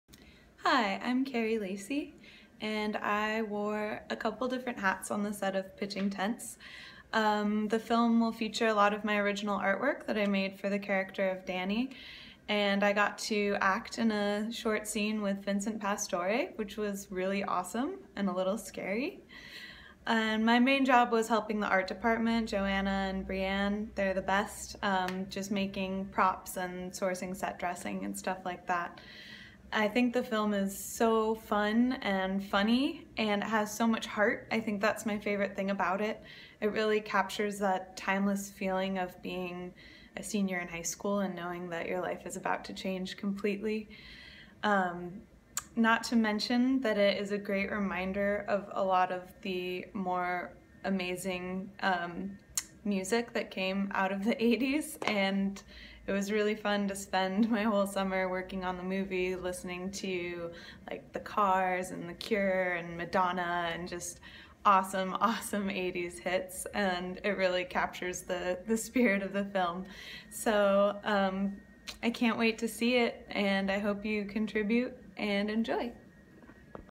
Kerry Lacy Shouts Out for Pitching Tents Movie!
Kerry Lacy's illustrations help define our lead character's talent as an artist.